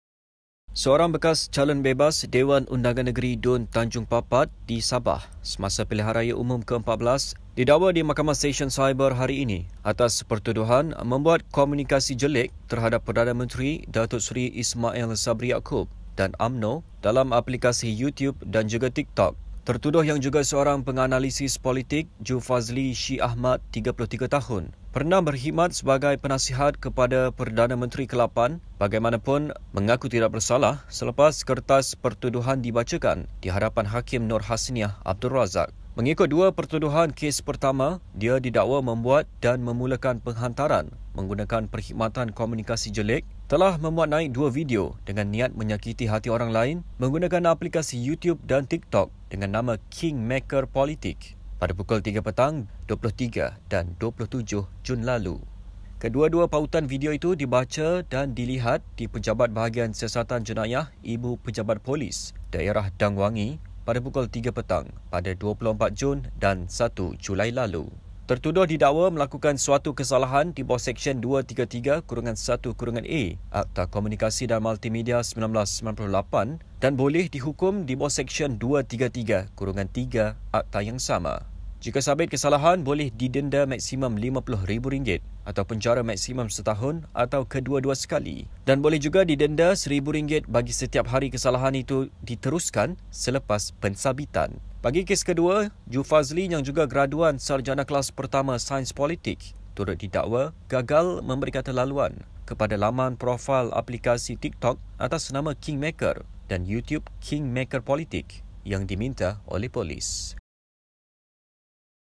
MGNews: Jufazli Mengaku Tidak Bersalah Hina Perdana Menteri
Seorang bekas calon bebas Dewan Undangan Negeri (DUN) Tanjung Papat di Sabah semasa Pilihan Raya Umum Ke-14 telah didakwa di Mahkamah Sesyen Siber di sini hari ini atas pertuduhan membuat komunikasi jelik terhadap Perdana Menteri, Datuk Seri Ismail Sabri Yaakob dan UMNO dalam aplikasi Youtube dan juga Tiktok.

Orang Kena Saman (OKS)  yang juga seorang penganalisis politik, Jufazli Shi Ahmad, 33, juga pernah berkhidmat sebagai penasihat kepada Perdana Menteri Kelapan itu bagaimanapun mengaku tidak bersalah selepas kertas pertuduhan dibacakan oleh jurubahasa di hadapan Hakim Nor Hasniah Ab Razak.